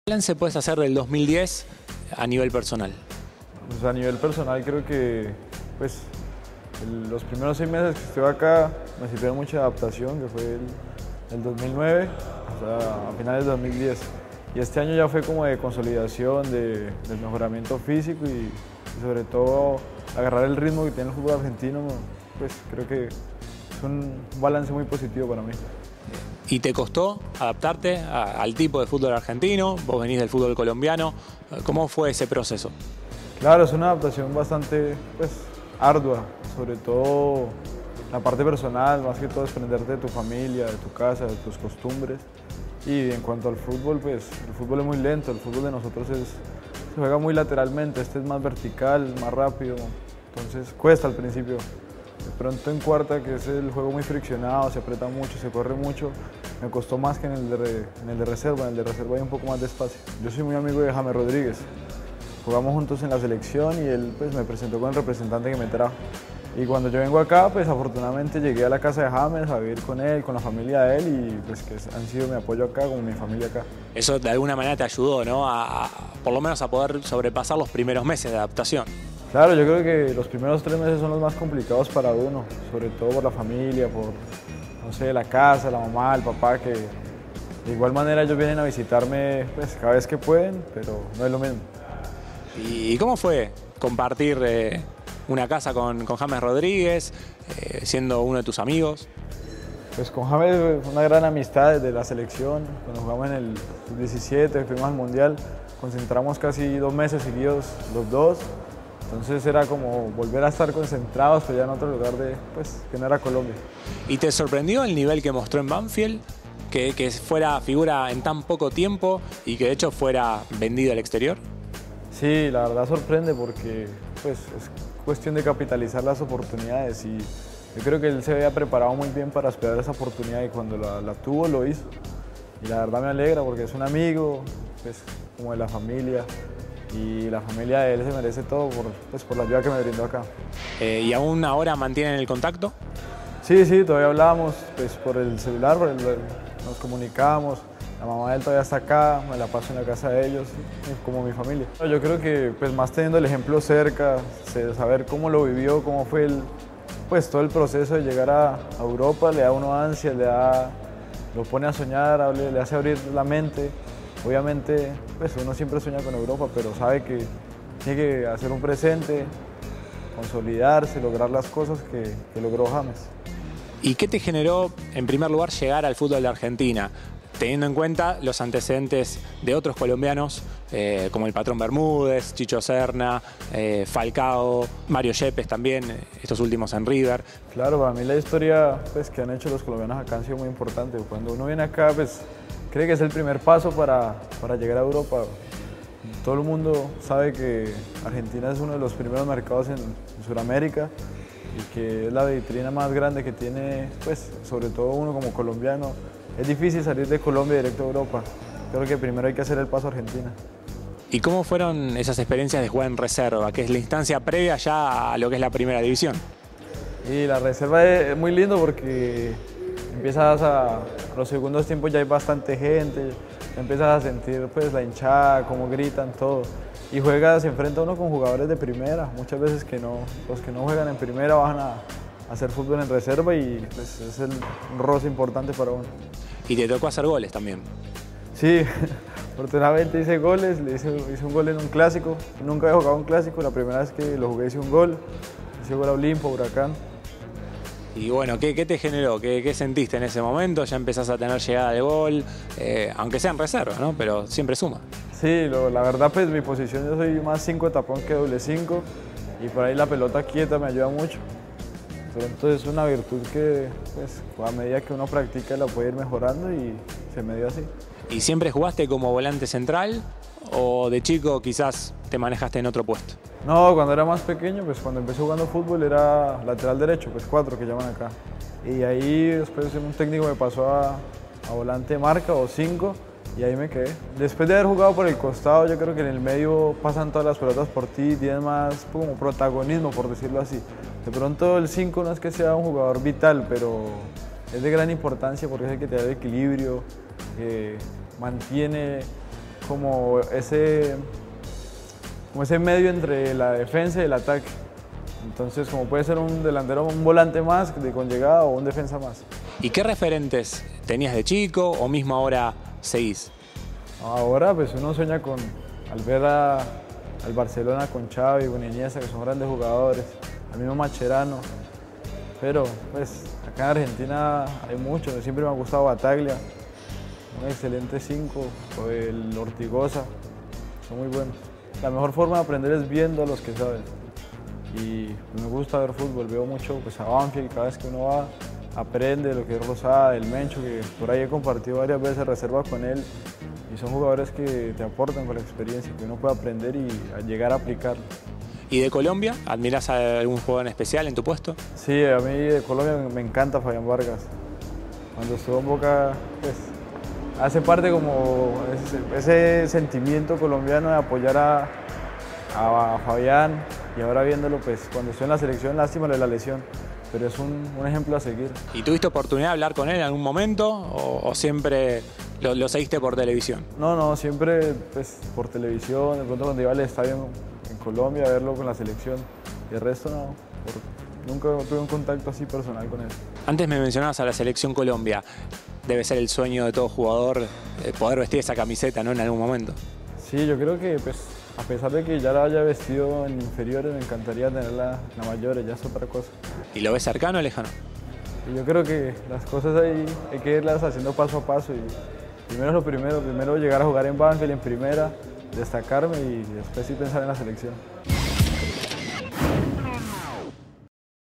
Julian Guillermo quiere triunfar en Banfield
El volante colombiano cuenta como fue su experiencia en reserva y afirma querer triunfar con la casaca del taladro.